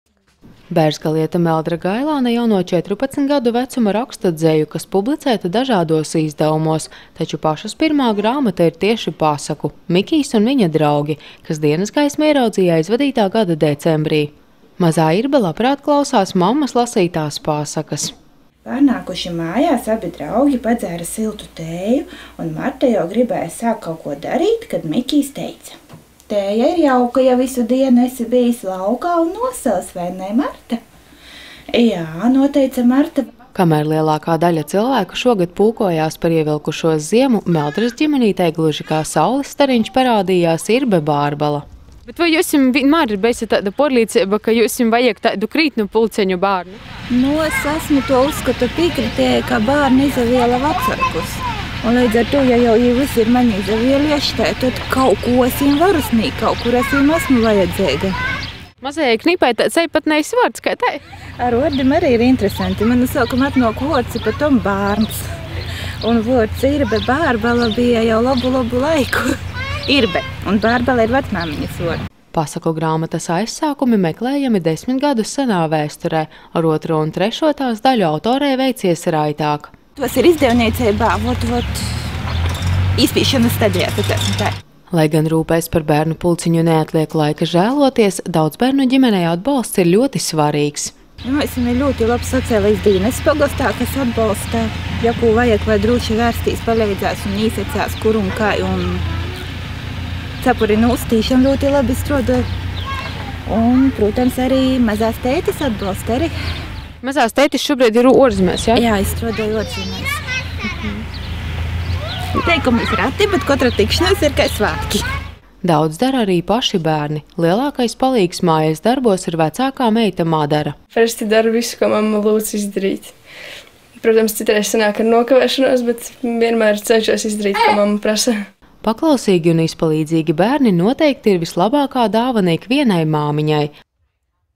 Piecu bērnu māmiņa raksta dzeju
Tradicionāli, maija otrajā svētdienā ik gadu tiek svinēti pavasara siltākie un sirsnīgākie svētki- Māmiņdiena. Ar puķēm, pašu zīmētām kartiņām un dzejolīšiem, vai kādu mīļu vārdu bērni priecē savas mammas un pateicas par rūpēm, kuras, kā atzīst 5cu bērnu māmiņa, bērzgaliete Meldra Gailāne, aizēno mīlestība pret savām atvasēm. Pateicoties tai klajā nākusi arī rakstnieces pasaku grāmata.